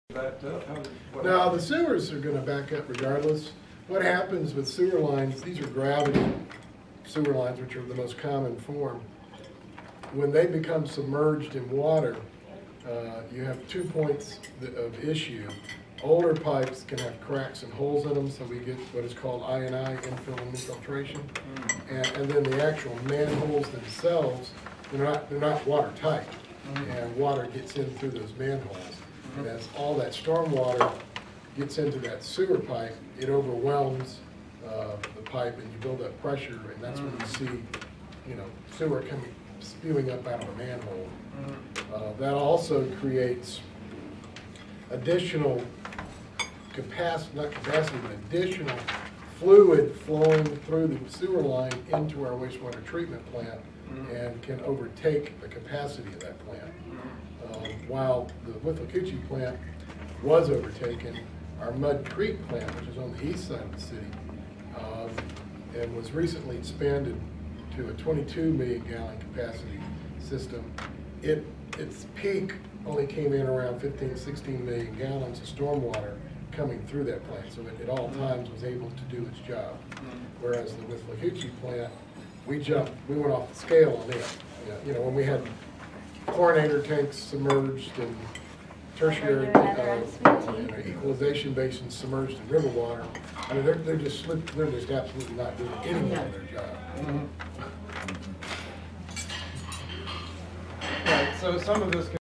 Sewer line problems @ WWALS 2013-03-13
Reservoirs for control of stormwater --Tim Carroll, Valdosta City Council
WWALS monthly board meeting, Adel, Cook County, Georgia, 13 March 2013.